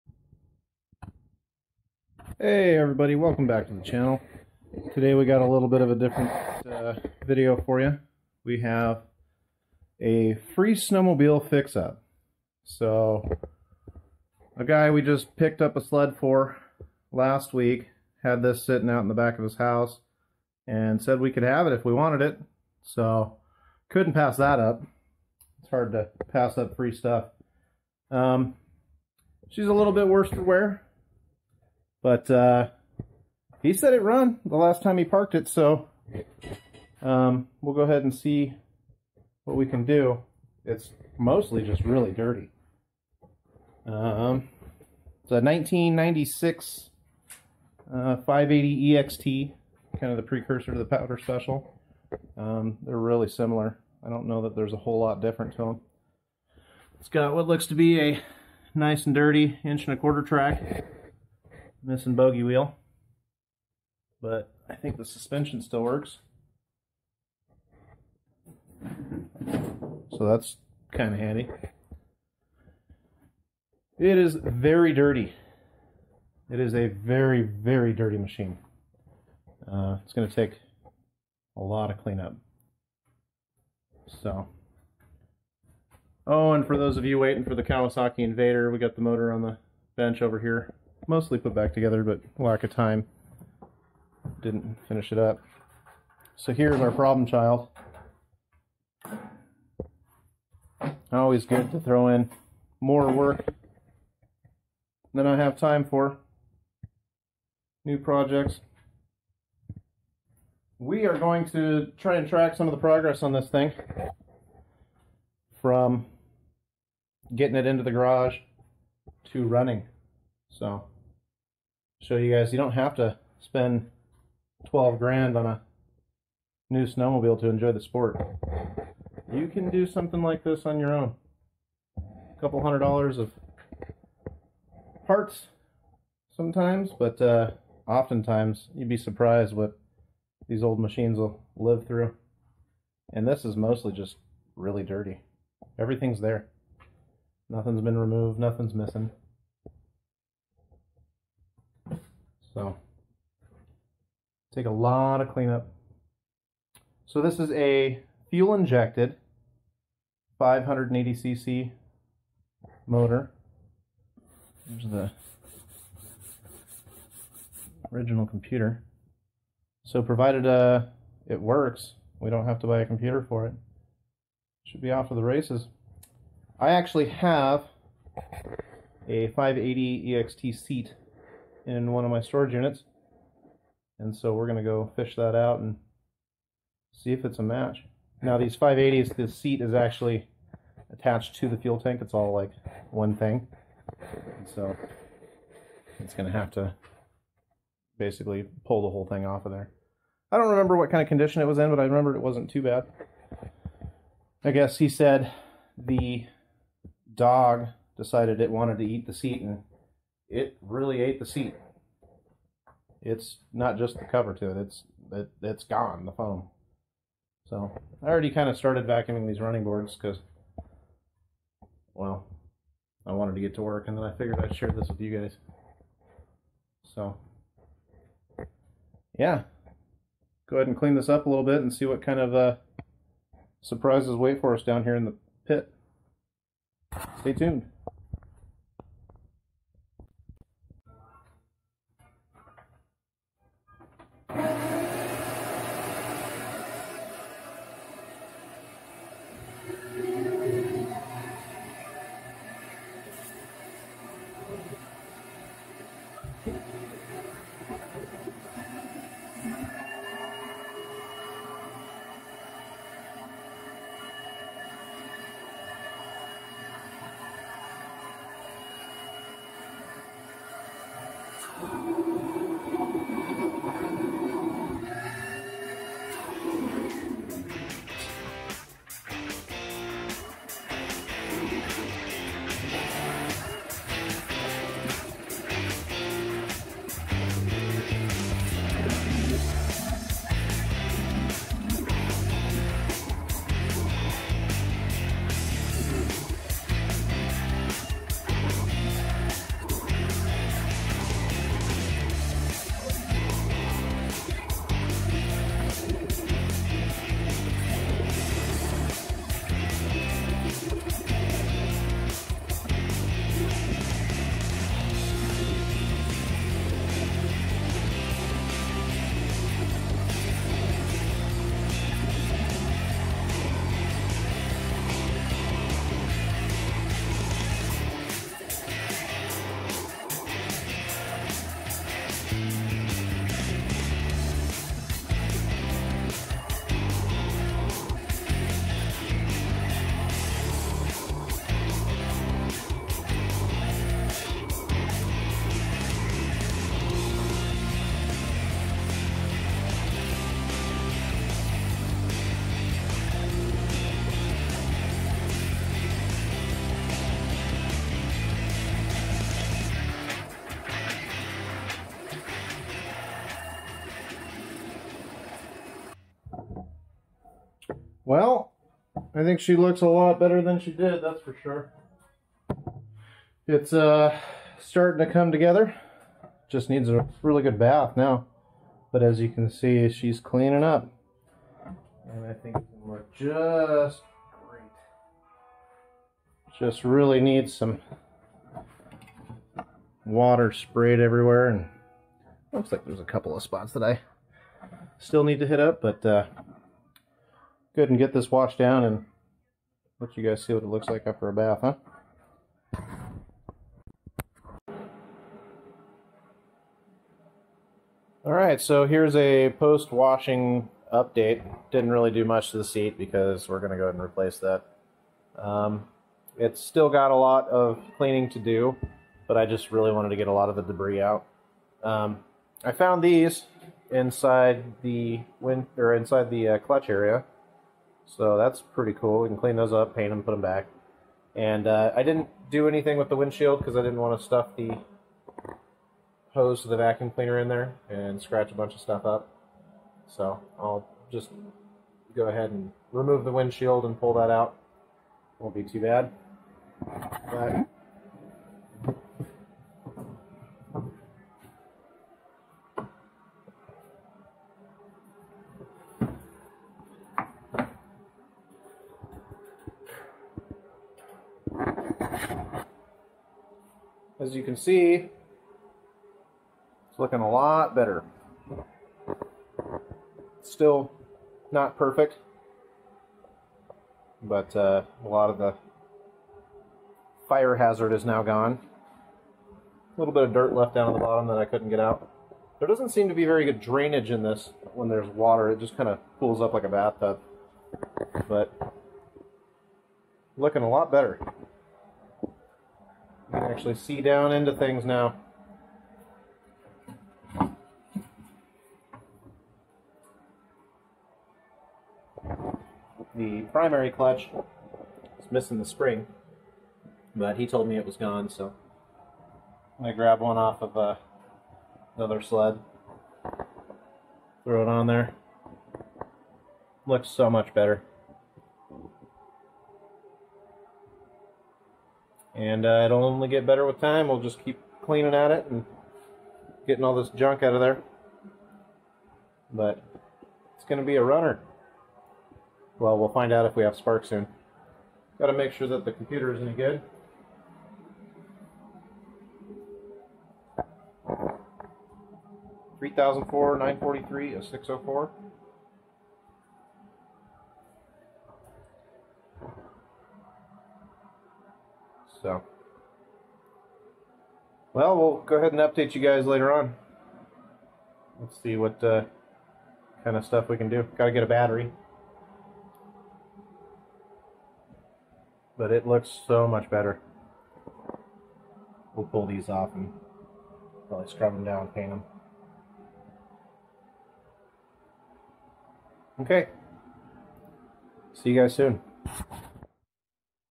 1996 580EXT Will it run?
So we picked up another snowmobile to add to the fleet.  This weeks vintage is a 1996 Arctic Cat EXT 580.  These were the predecessor to the Powder Special that they made exactly 56 trillion of. Fact.  So this little beauty has been resting surrounded by cotton wood trees and wasn't covered. We should have our work cut out for us. Hopefully we can make some progress and get it running!